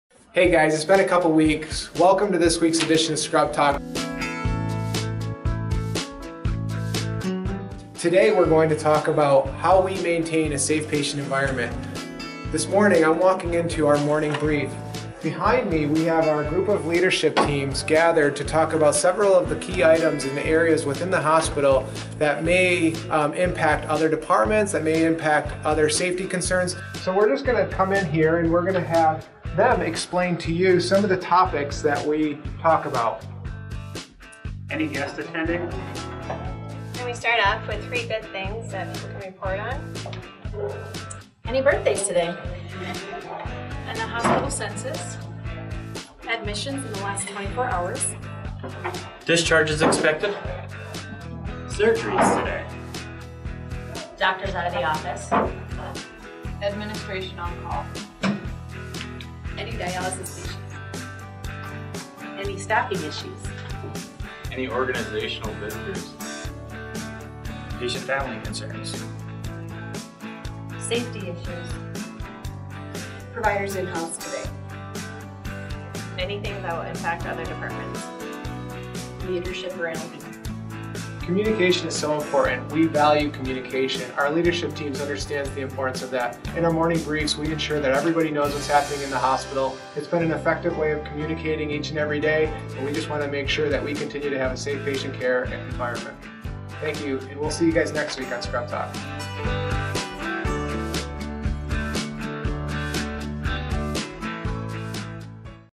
Scrub Talk - Morning Huddles
The leadership team discusses a small strategy that makes a big impact on all departments. Each day the hospital leaders communicate important topics in a brief, yet effective, morning meeting.